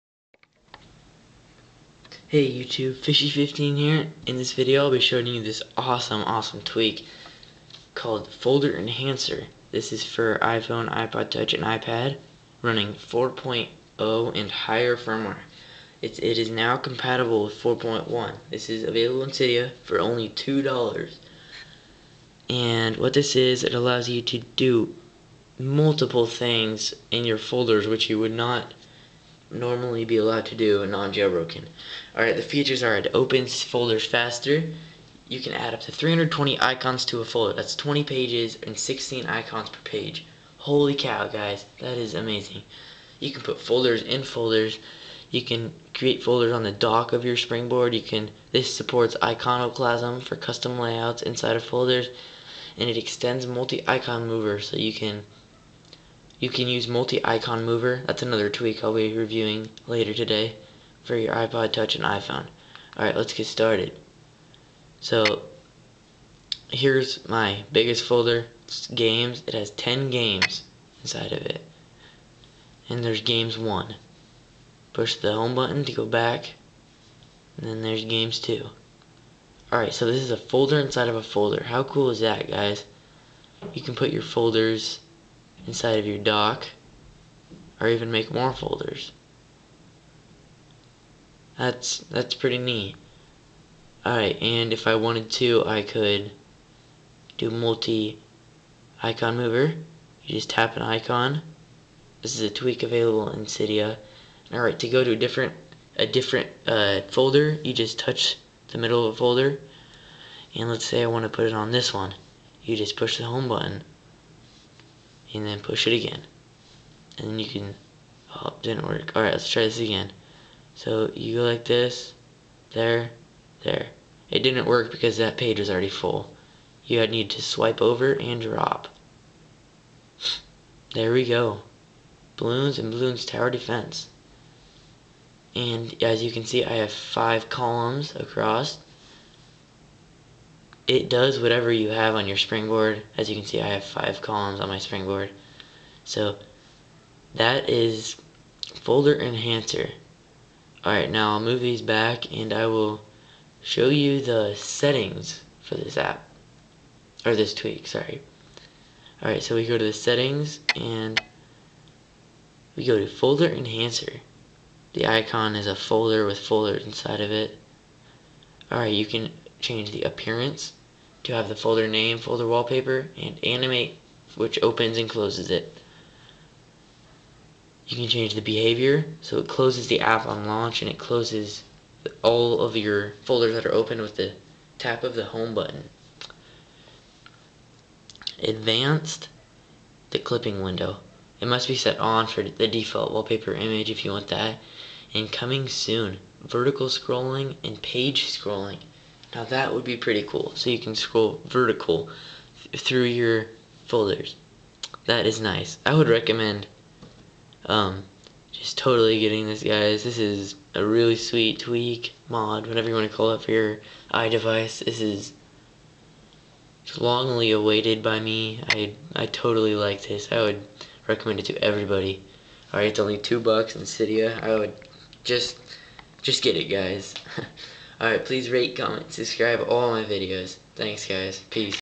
FolderEnhancer iPhone, iPad and iPod Touch Tweak! (iOS 4.x)
Available in Cydia for $1.99

Features:
-Open folders faster
-Add up to 320 icons to a folder (20 pages, 16 per page)
-Create folders inside of folders
-Create folders in the dock
-Supports iconoclasm for custom layouts!
-Allows you to use MultiIconMover
-Options include hiding the folder name, wallpaper, as well as disabling animations!

Now avaliable for iOS 4.1! Supports all devices with iOS 4.0 or higher.